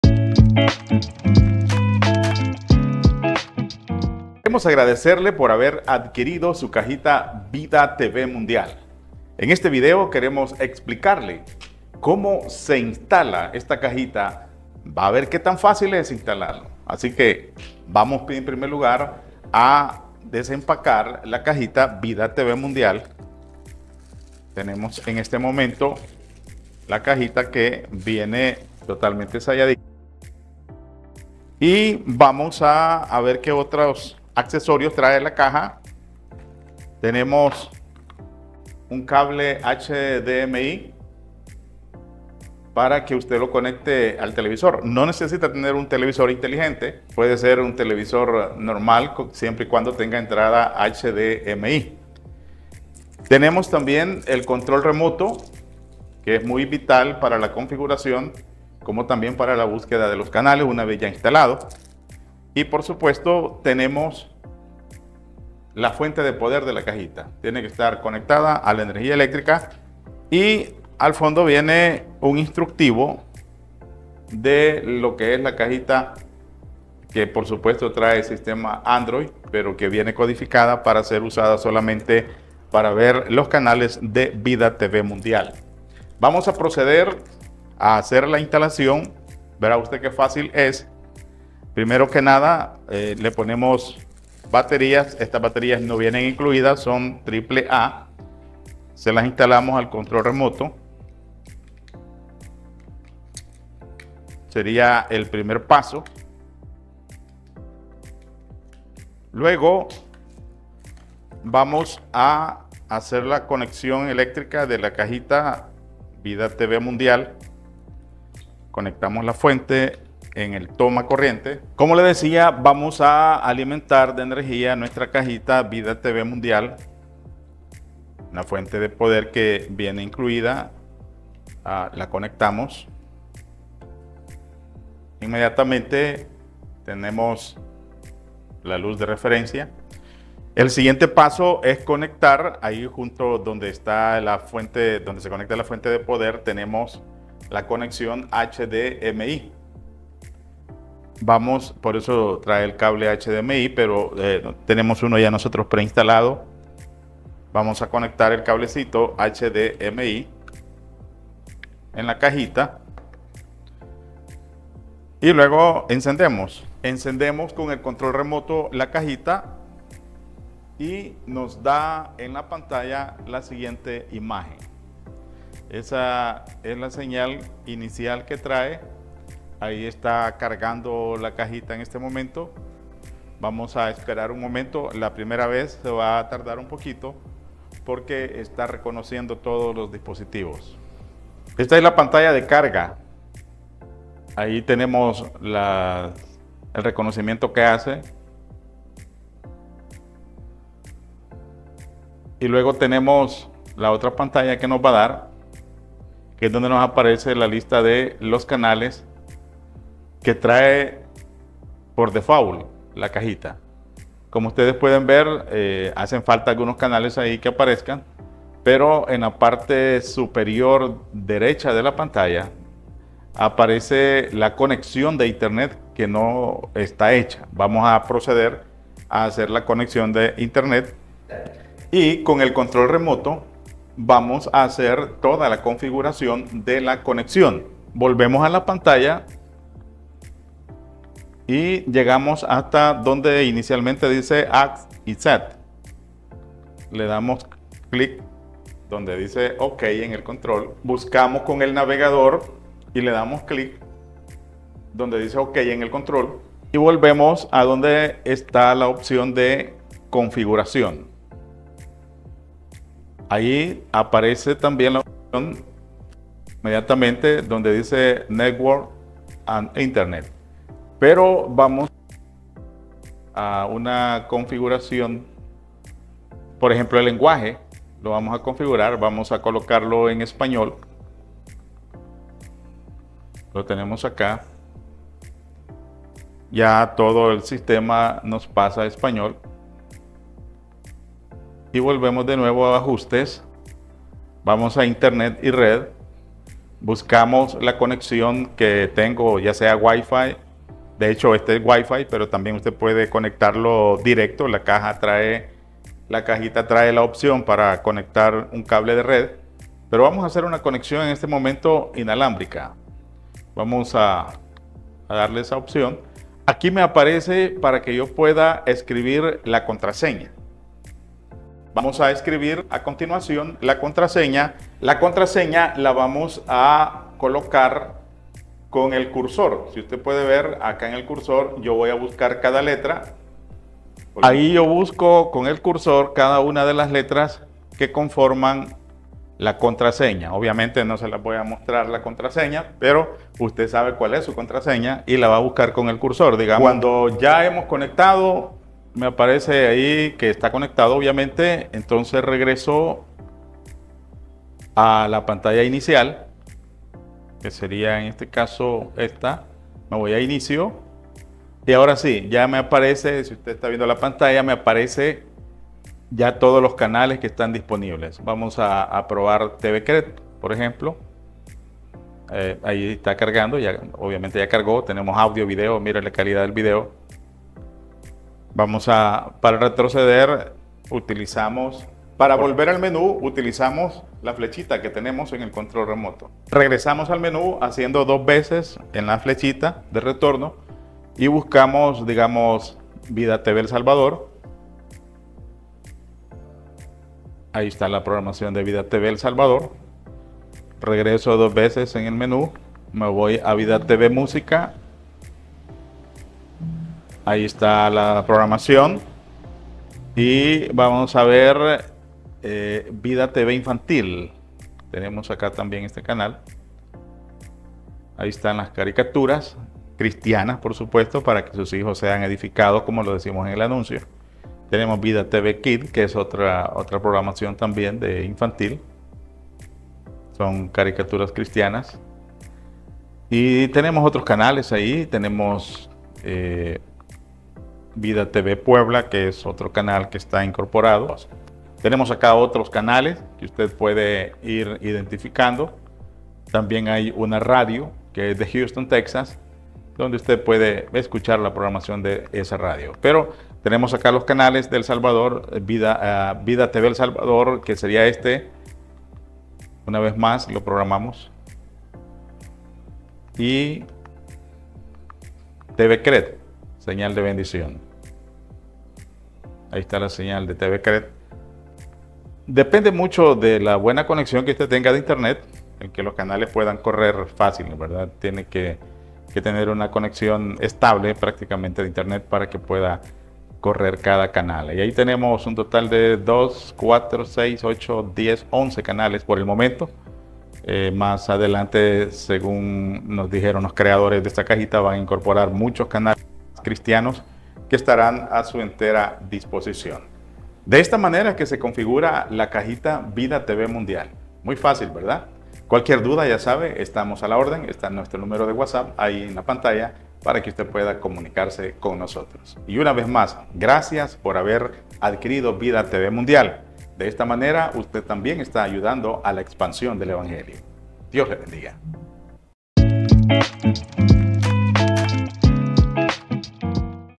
Como instalar la TV box Vida Tv Mundial
Aquí le explicamos paso a paso sobre como hacer la instalación de la cajita Vida Tv Mjndial